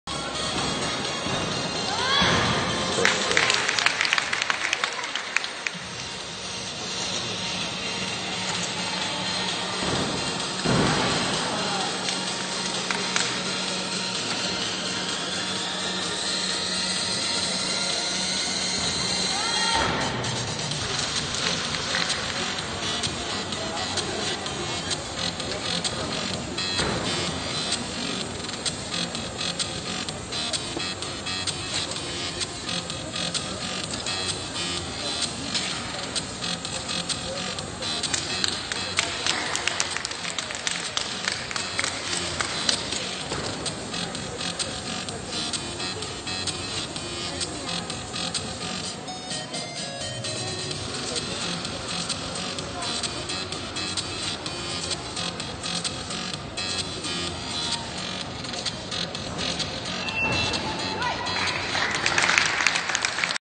MAMENOV Roman (KAZ) - FX - Voronin Cup 2019
Маменов Роман - Казахстан - вольные упражнения - Кубок Михаила Воронина 2019 - г.Пенза
D: 4.5
TS: 7.3
Place in final: 8